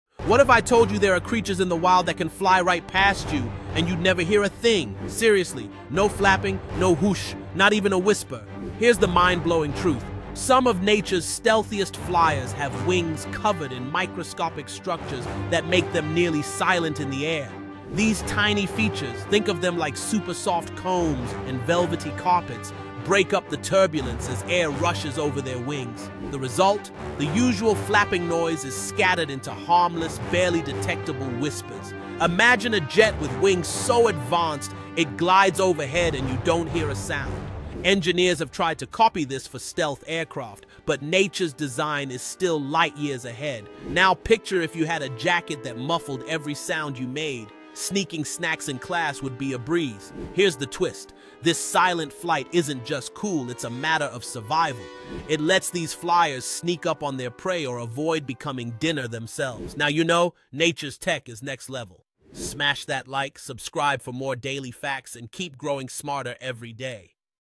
How Do Some Birds Fly Without a Sound?
Ever wonder how some creatures fly in total silence? Discover the mind-blowing science behind invisible flight!
Ever wonder how some birds achieve silent flight? 🦉 In this video, we explore the incredible adaptations of super powered owls and their unique wing structures that allow for stealthy movement through the skies. Join us for stunning nature footage that showcases the fascinating behavior of these majestic wildlife creatures!